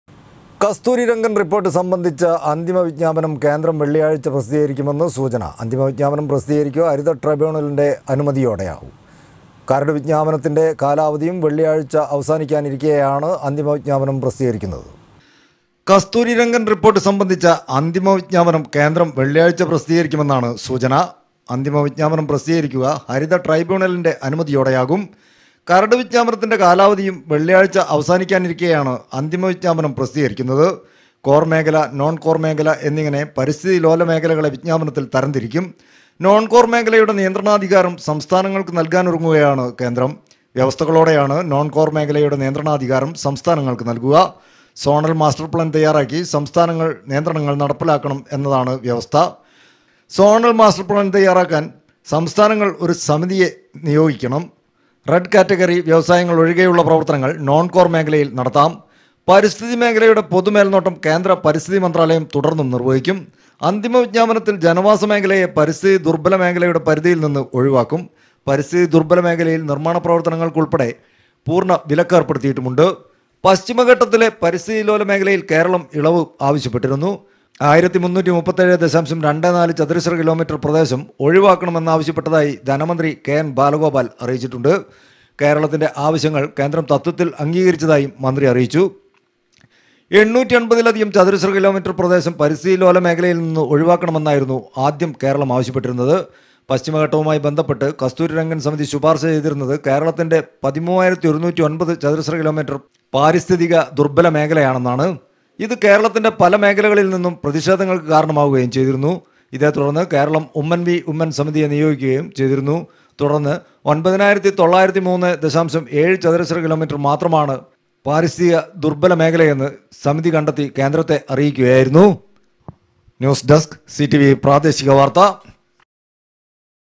കസ്തൂരിരംഗൻ റിപ്പോർട്ട് സംബന്ധിച്ച അന്തിമ വിജ്ഞാപനം കേന്ദ്രം വെള്ളിയാഴ്ച പ്രസിദ്ധീകരിക്കുമെന്ന് സൂചന
കസ്തൂരി രംഗൻ റിപ്പോർട്ട് സംബന്ധിച്ച അന്തിമ വിജ്ഞാപനം കേന്ദ്രം വെള്ളിയാഴ്ച പ്രസിദ്ധീകരിക്കുമെന്ന് സൂചന. അന്തിമ വിജ്ഞാപനം പ്രസിദ്ധീകരിക്കുക ഹരിത ട്രൈബ്യൂണലിന്റെ അനുമതിയോടെയാകും. കരട് വിജ്ഞാപനത്തിന്റെ കാലാവധിയും  വെള്ളിയാഴ്ച അവസാനിക്കാനിരിക്കെയാണ് അന്തിമ വിജ്ഞാപനം  പ്രസിദ്ധീകരിക്കുന്നത്.